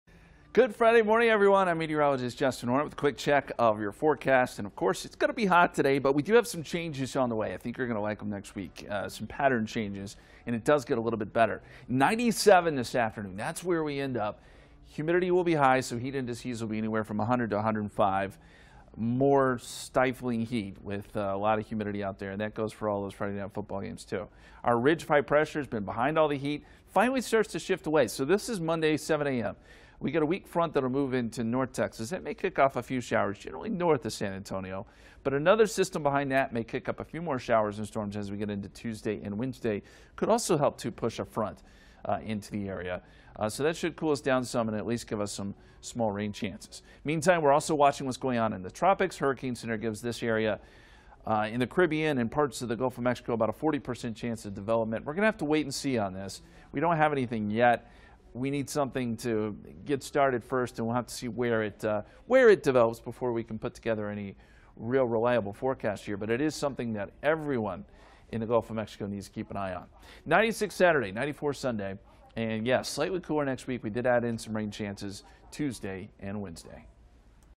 Weekend forecast, plus a peek into next week
Temperatures finally drop a bit and we add in rain chances next week.